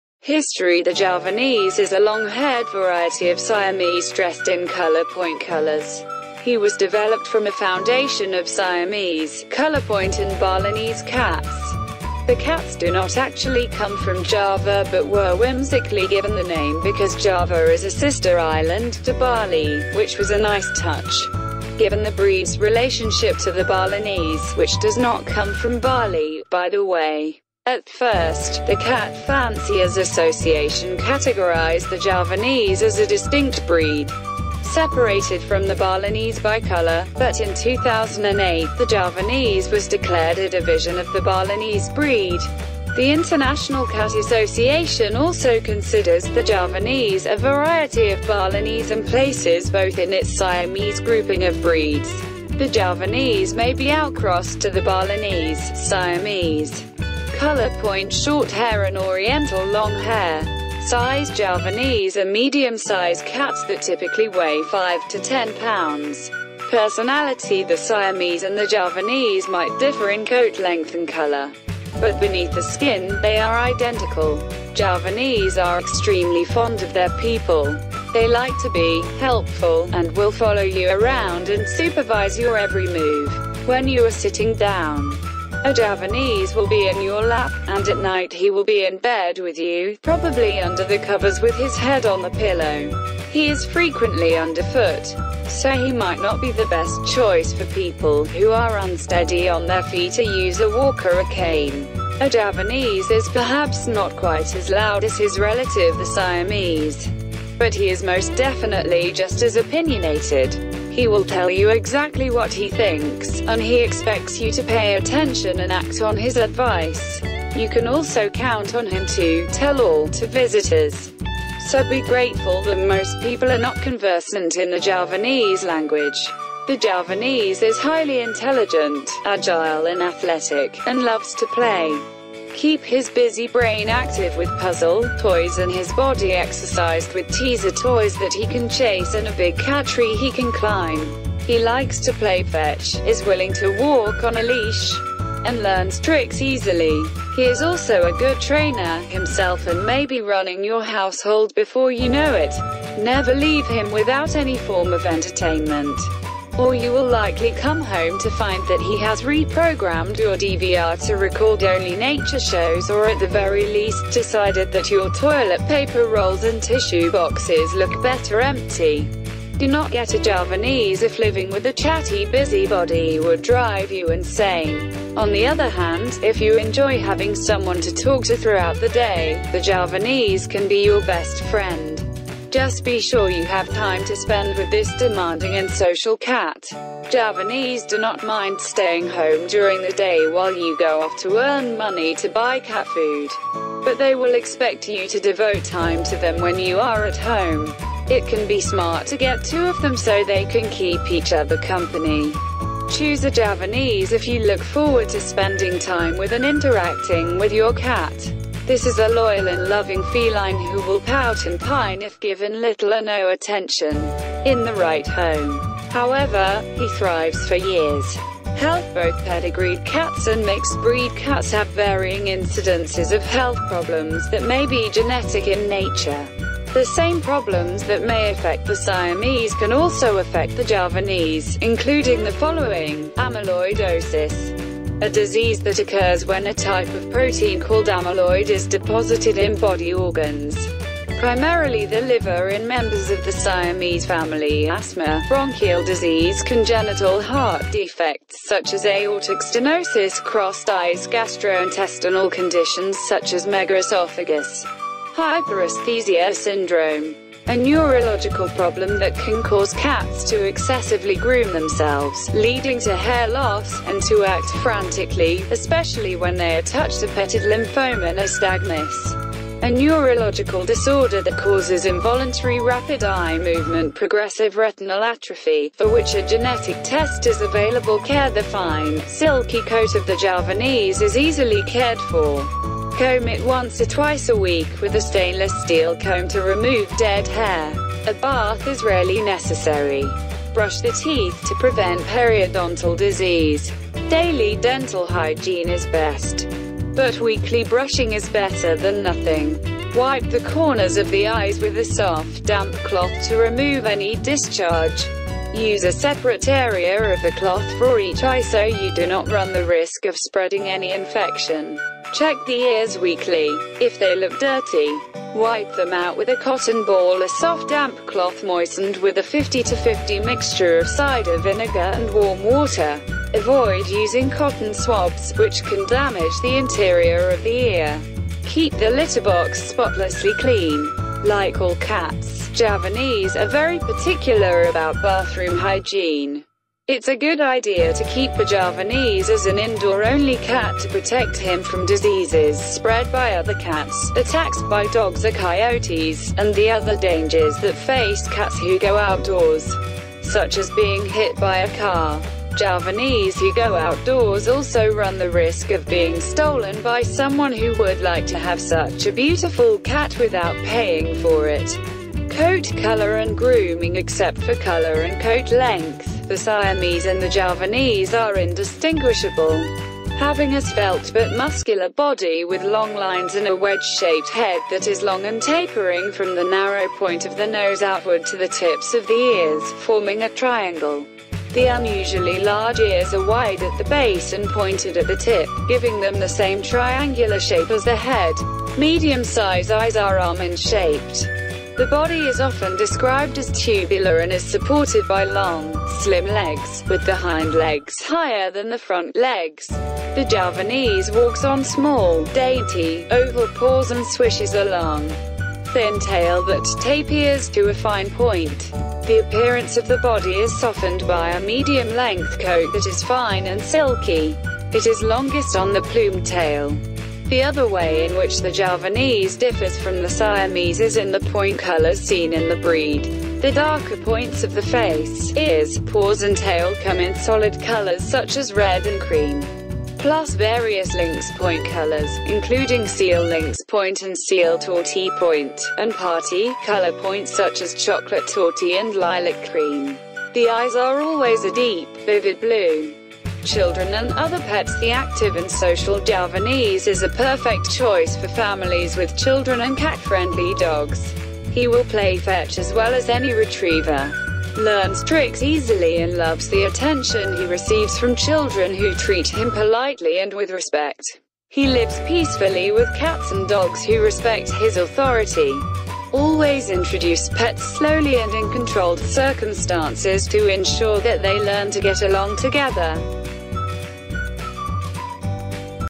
Javanese Cat, History, Personality, Coat Color And Grooming
History

The Javanese is a longhaired variety of Siamese dressed in Colorpoint colors. He was developed from a foundation of Siamese, Colorpoint and Balinese cats. The cats do not actually come from Java but were whimsically given the name because Java is a sister island to Bali, which was a nice touch, given the breed’s relationship to the Balinese (which does not come from Bali, by the way). At first, the Cat Fanciers Association categorized the Javanese as a distinct breed, separated from the Balinese by color, but in 2008 the Javanese was declared a division of the Balinese breed. The International Cat Association also considers the Javanese a variety of Balinese and places both in its Siamese grouping of breeds. The Javanese may be outcrossed to the Balinese, Siamese, Colorpoint Shorthair and Oriental Longhair.